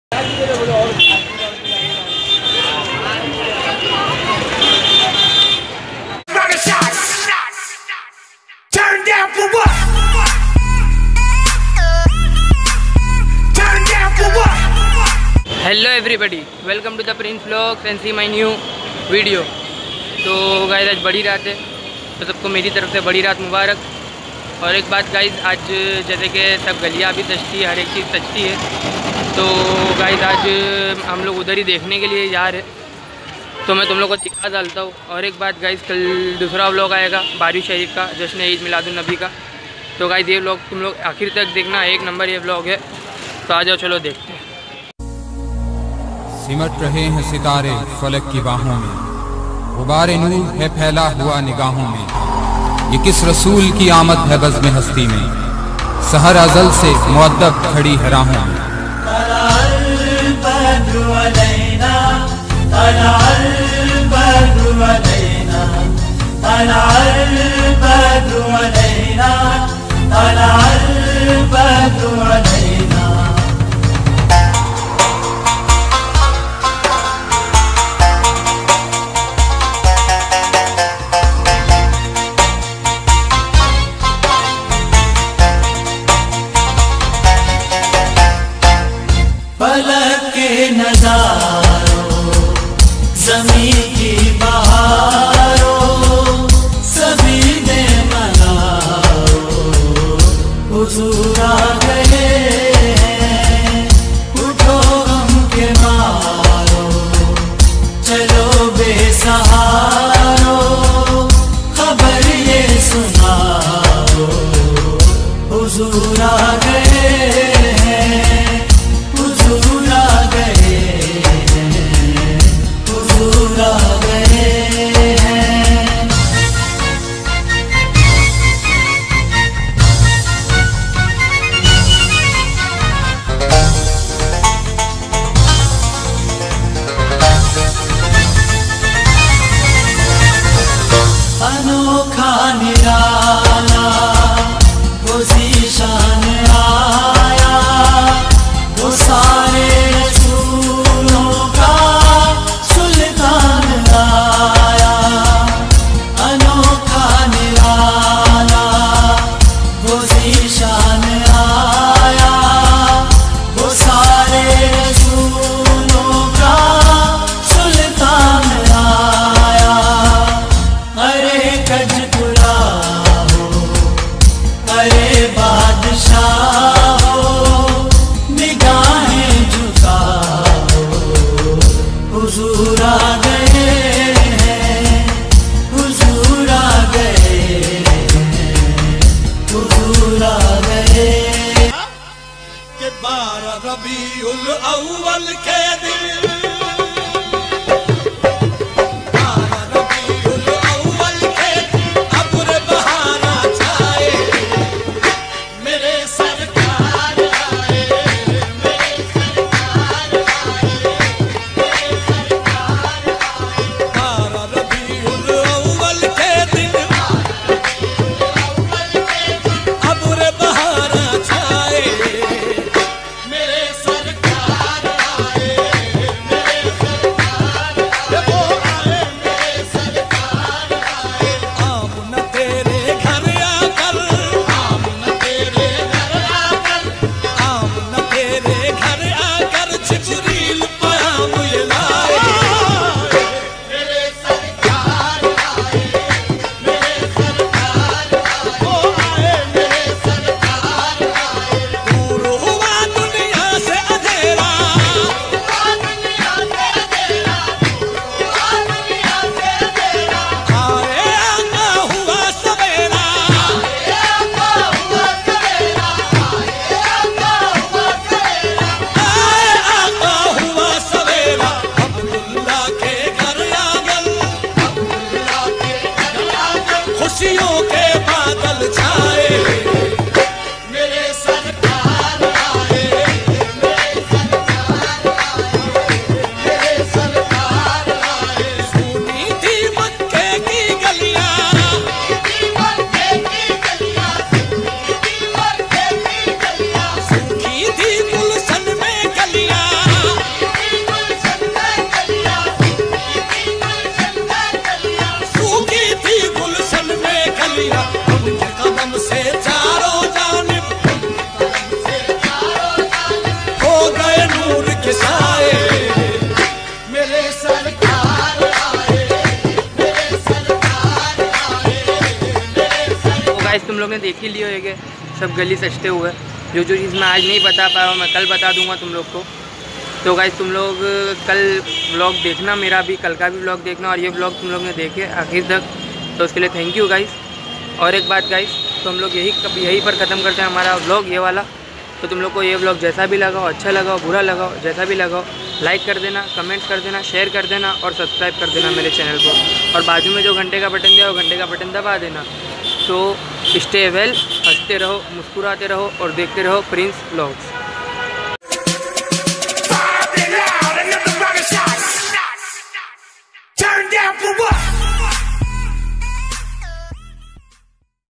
CHAAND RAAT CELEBRATION IN MUMBAI❤❤❤ ||PRINCE VLOG'S
CHAAND RAAT MUBARAK TO AAL MY BROTHER SISTER AND ALL MY RELATIVES  IN THIS DAY ALL GALLI AND MOHALLAH HAS DECORATED BY CHIRAAG AND LIGHTINGS AND ETC THINGS HAS DECORATED

YOU WILL LIKE THIS VLOG

Add me on Snapchat! Username: princehr786